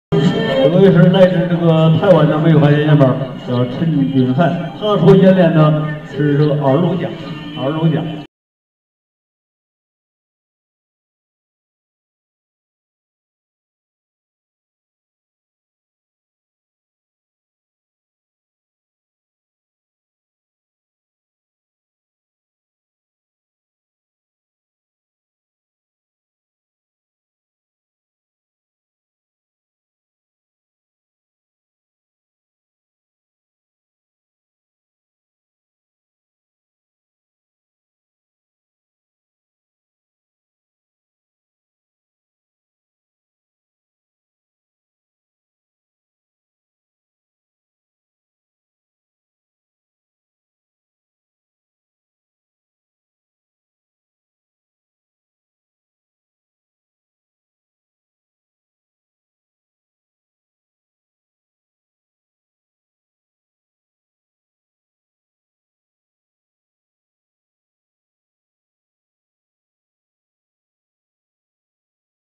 Mei hua quan lao jia 梅花拳老架
Sequenza Lao jia 老架 (“Struttura antica”) del mei hua quan 梅花拳 (“pugilato del fior di prugno”), praticata da Xu Ming Xing a Leijiacun 雷家村 (“Villaggio della Famiglia Lei”).